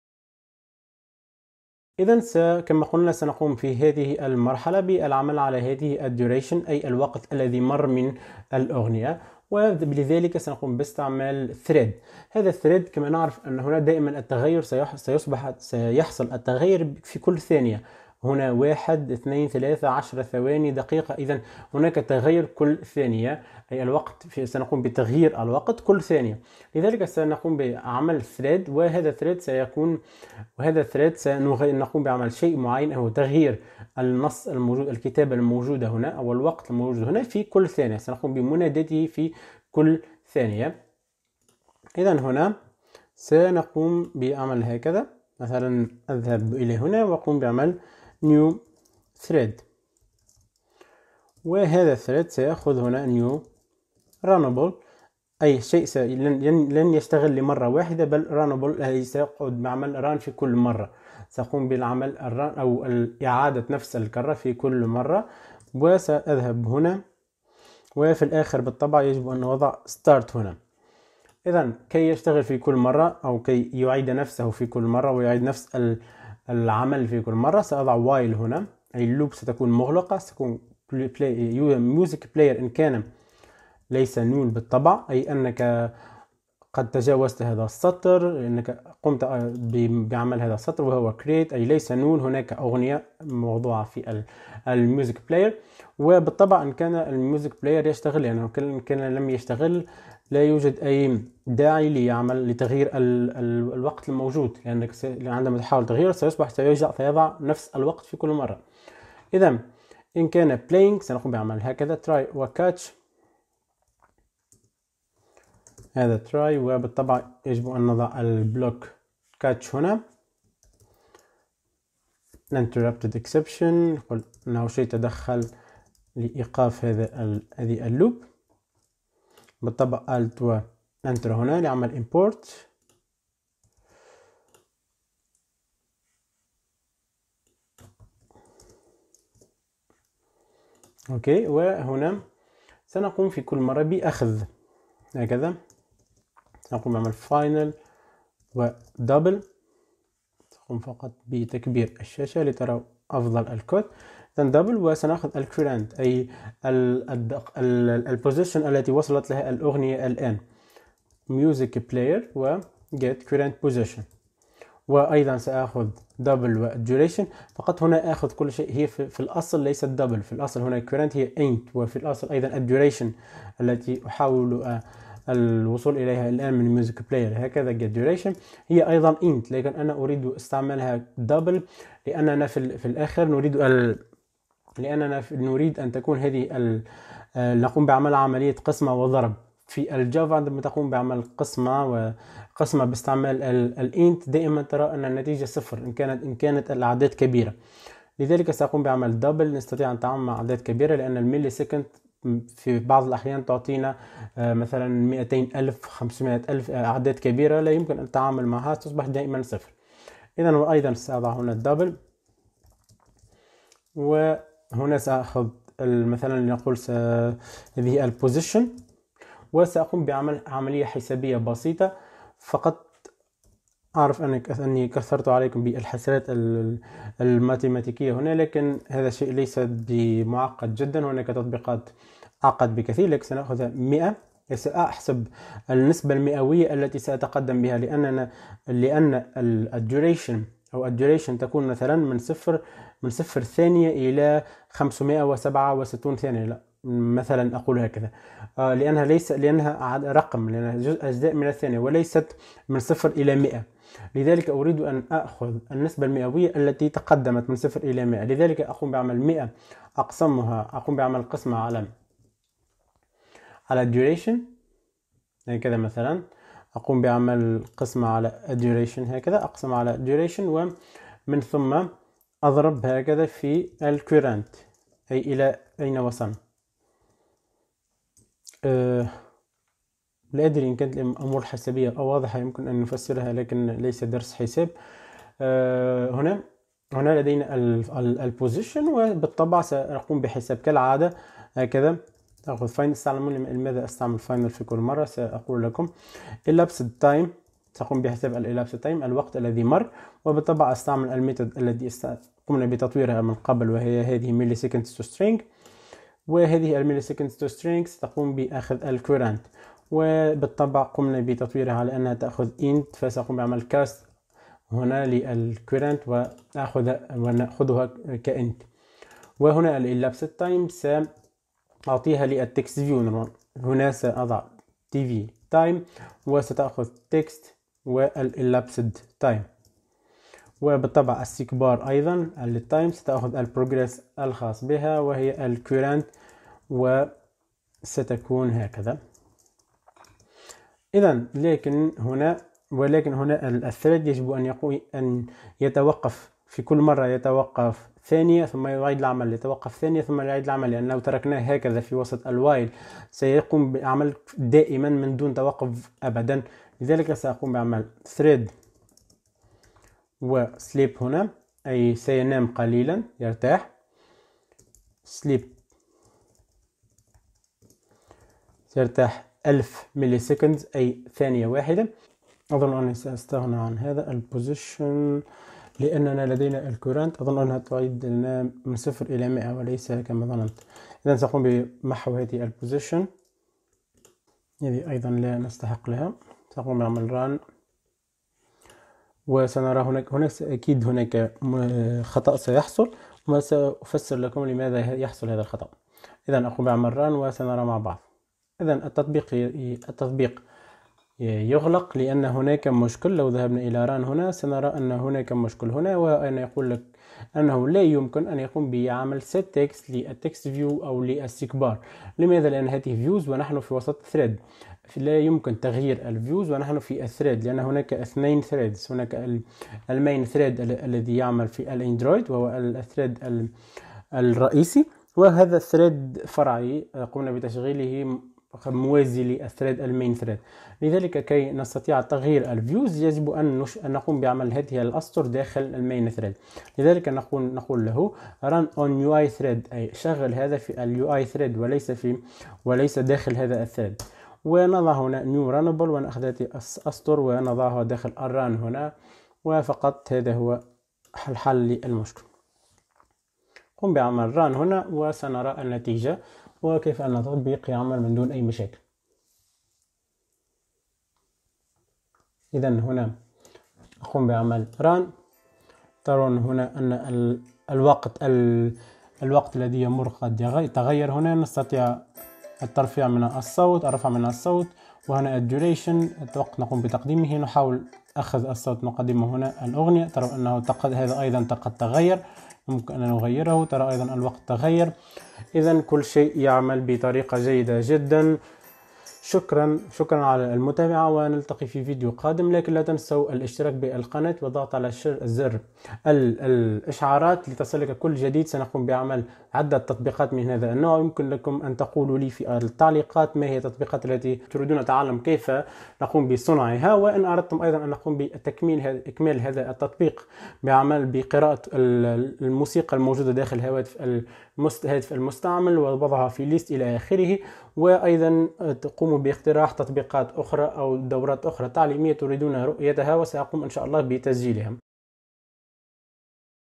#5 Create Music Player Tutorial - Android Studio | RunOnUiThread & Thread | تطبيق مشغل أغاني
In this tutorial you will learn how to create music player App in Android Studio
We use Java for this Android Application and Android Studio.
- Android Studio
- SeekBar
- Threads
- runOnUiThread
- Gradient background drawable with xml
- Volume control
- MediaPlayer